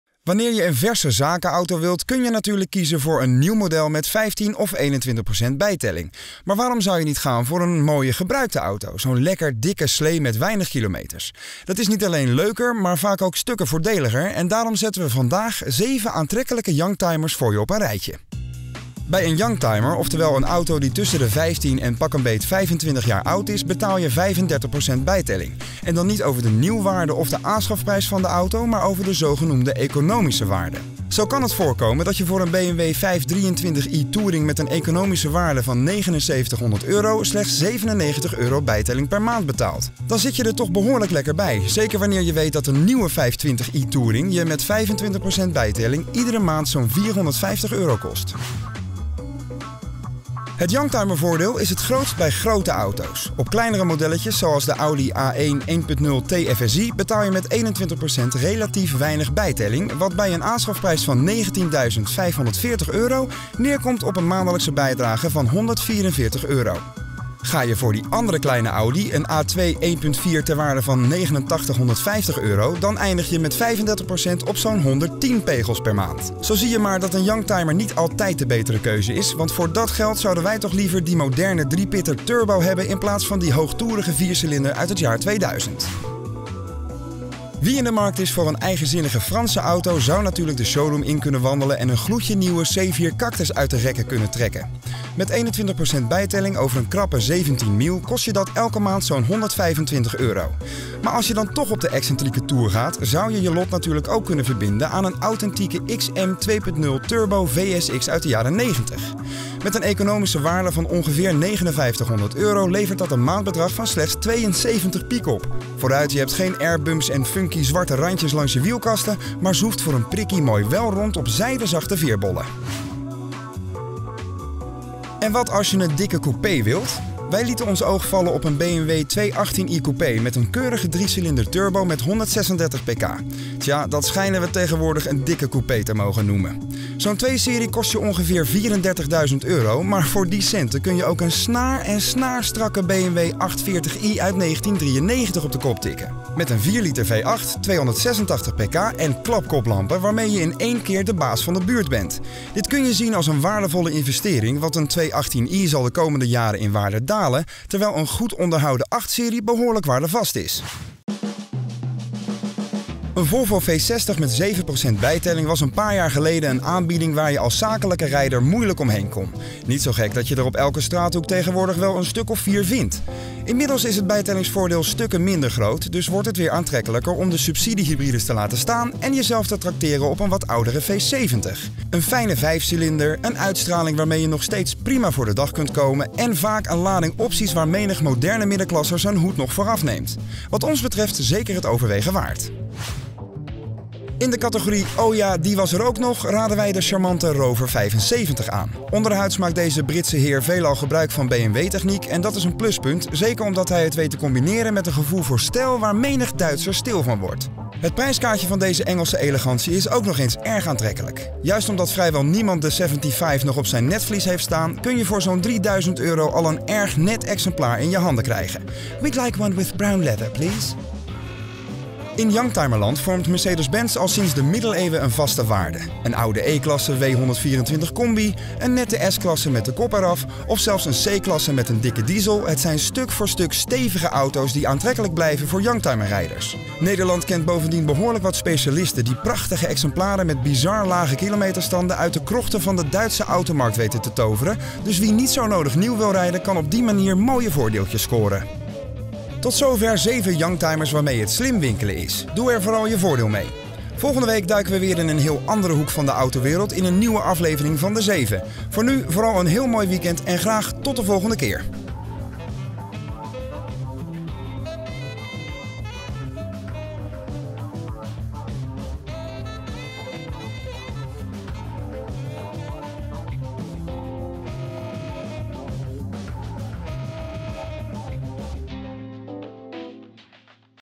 De 7 youngtimers voor slimme mensen
Een nieuwe auto is leuk, maar voor zakelijke rijders kan een youngtimer van 15 jaar en ouder minstens zo aantrekkelijk zijn. We selecteren vandaag zeven aantrekkelijke youngtimers.